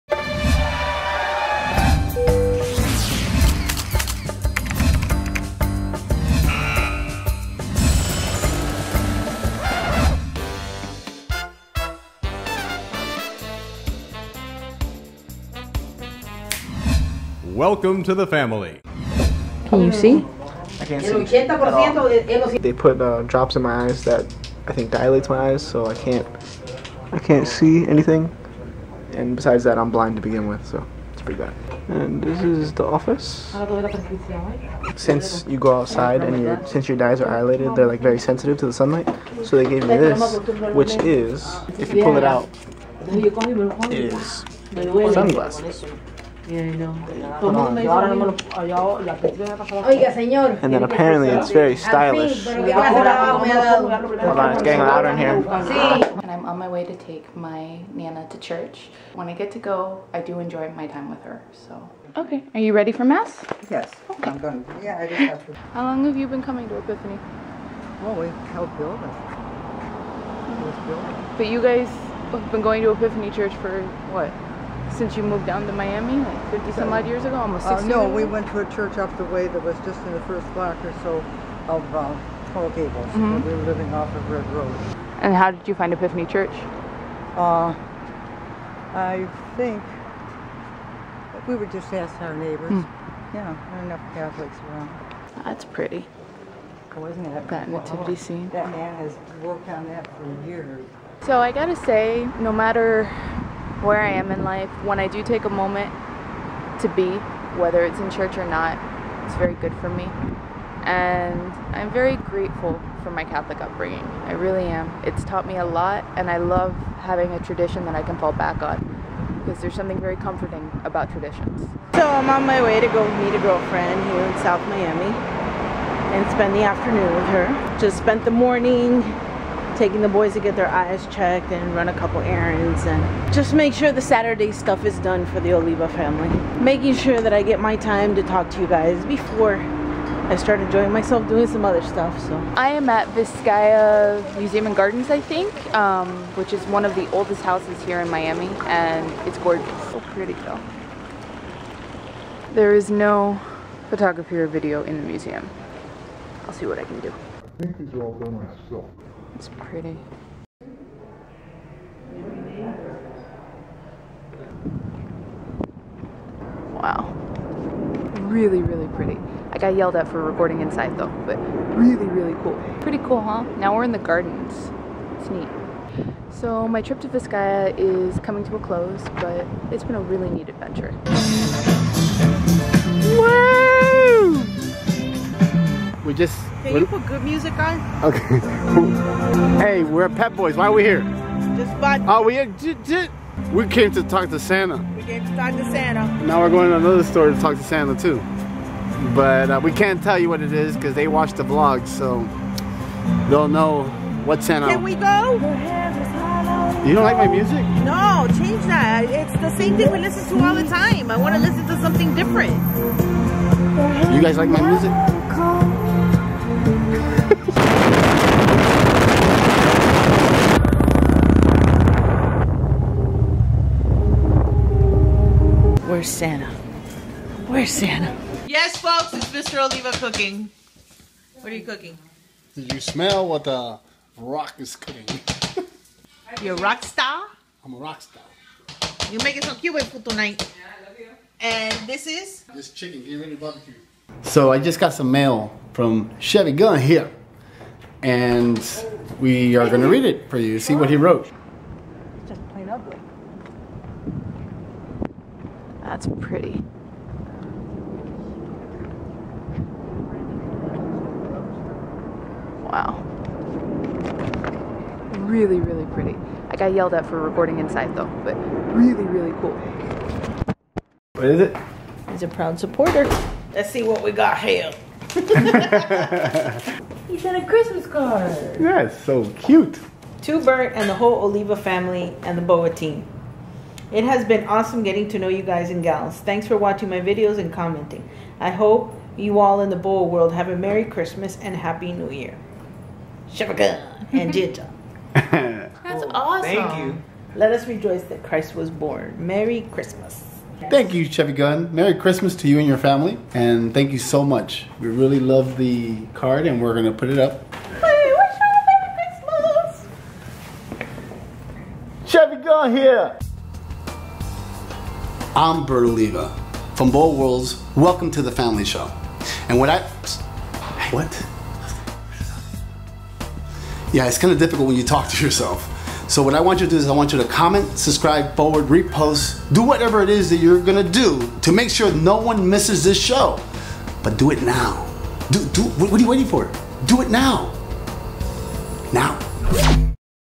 VIEW FROM VISCAYA (Season 2, Episode 11)
Who is blind? Who's talking to Santa? What can't you do at the Viscaya? Watch to find out!

The IT Wizard takes her grandmother out for a day of exploration as the Marketing Crusader makes sure her kids are focused! When the Motivational Mogul is caught in the kitchen the team is greeted by an unexpected message from a proud supporter.

BOWA Studios
12080 Southwest 127 Avenue
Suite 305